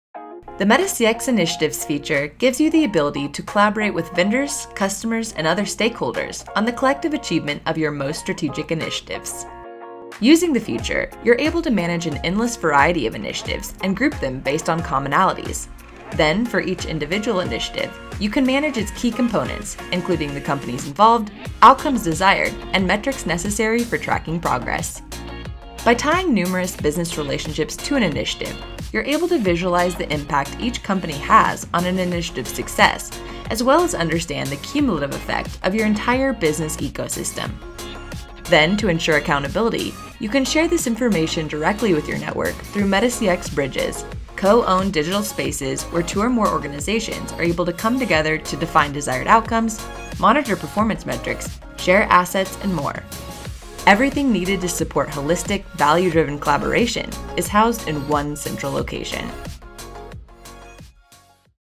Feature Overview: MetaCX Initiatives
The MetaCX initiatives feature gives you the ability to collaborate with vendors, customers, and other stakeholders on the collective achievement of your most strategic initiatives.